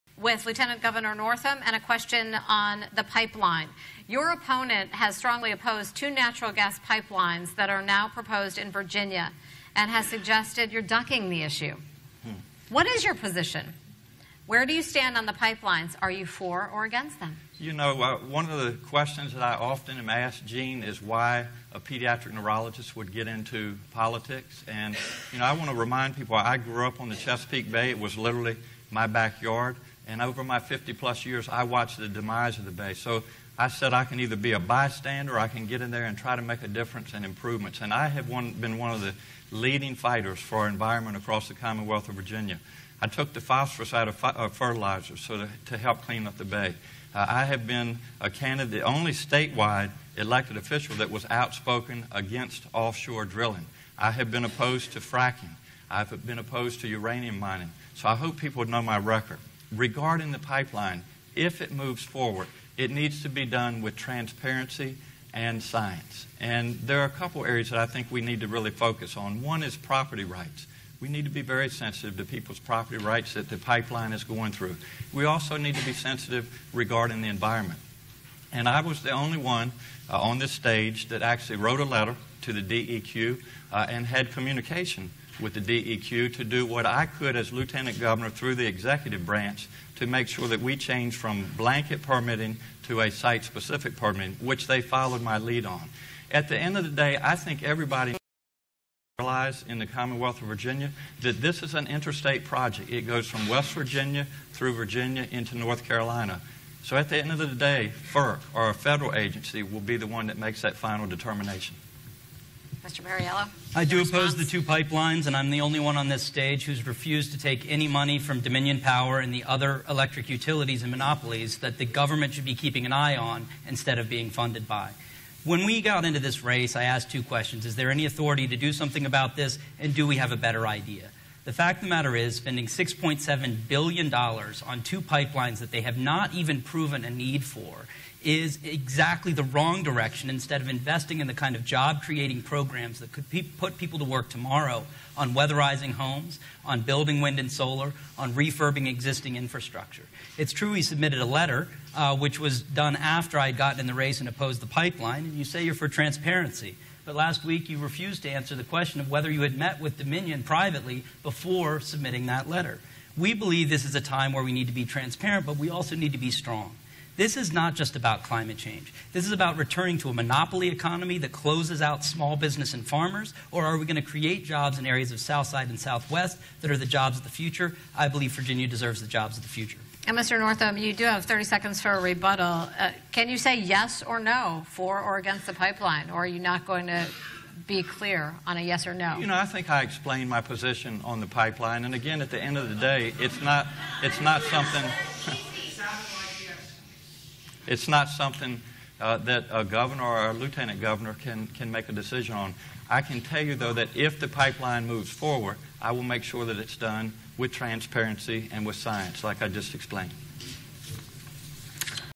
Ralph Northam Ducks Pipeline Question For The Third Straight Time
VA Dem Debate 5/4/2017